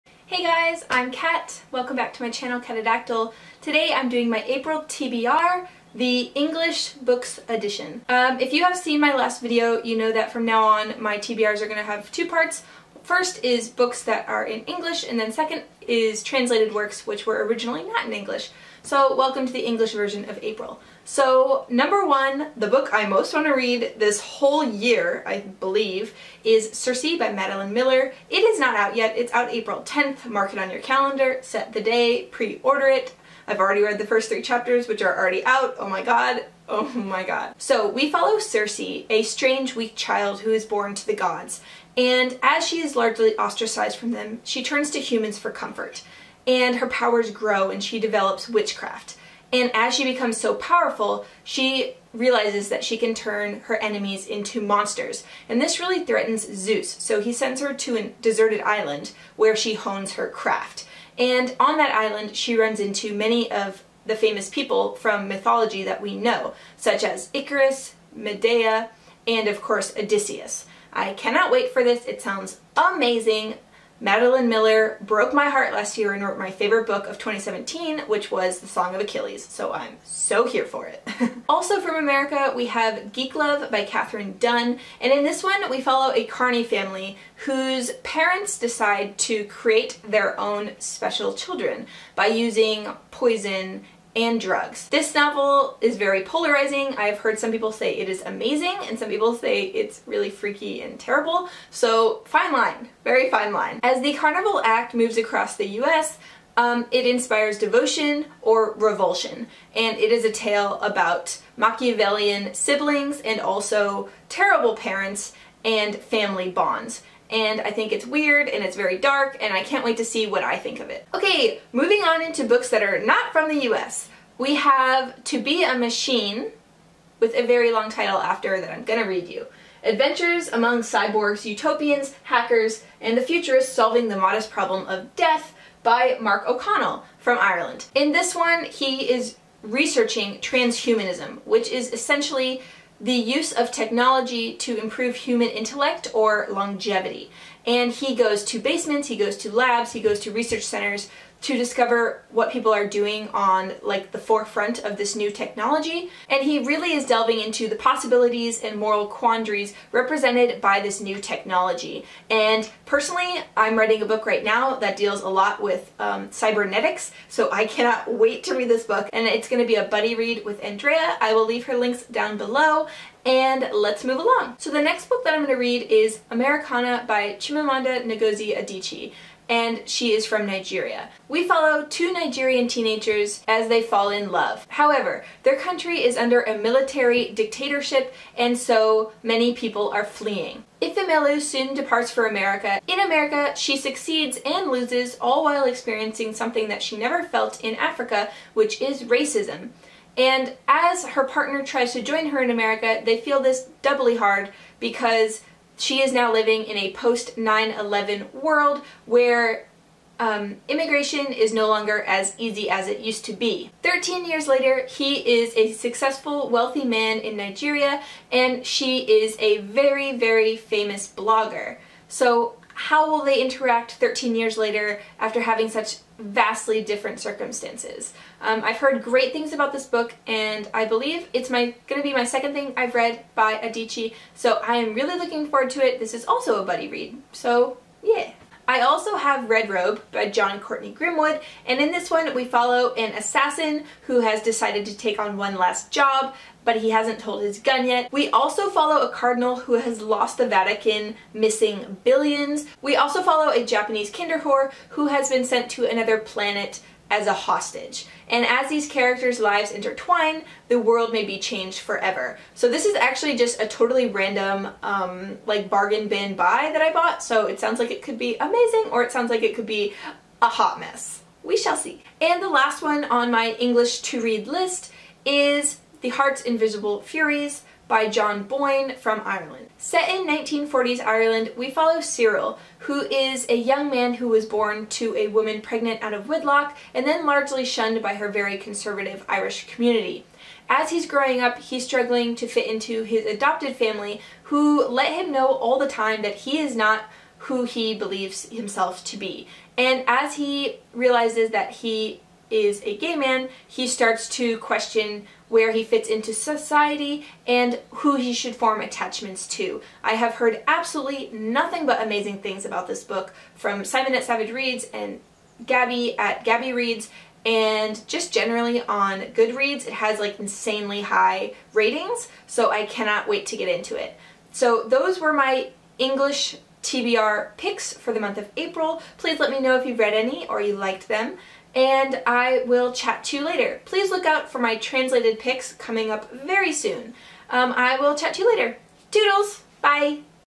English TBR | April 2018
Welcome to the second part of my April TBR! I'm excited to read so many of these books. From best-sellers to random bargain bin pickups, I hope they're all amazing :)

// people //

// possibly wonderful books //

Fun fact: The first 3 chapters are free on Kindle RIGHT NOW!!

Astrophysics for People in a Hurry - Neil deGrasse Tyson // (USA: Hiroshima Book Club's April pick)

To Be a Machine: Adventures Among Cyborgs, Utopians, Hackers, and the Futurists Solving the Modest Problem of Death - Mark O'Connell // (Ireland)